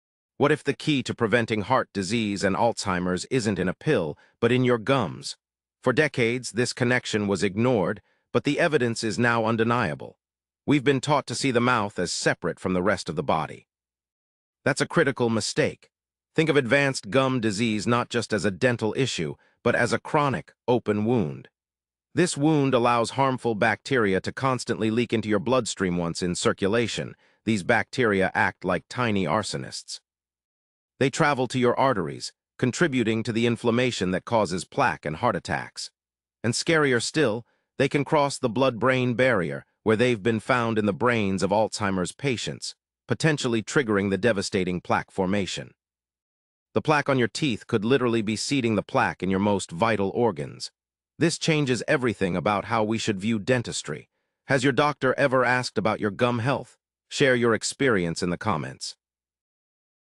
How Dirty Gums Can Hurt Your Whole Body!
Did you know there's a strong connection between your oral systemic link and overall well-being? New studies highlight how conditions like gingivitis and periodontitis can impact your brain, potentially increasing the risk of alzheimers disease. Maintaining good oral health, including controlling plaque, is more crucial than ever for dental health.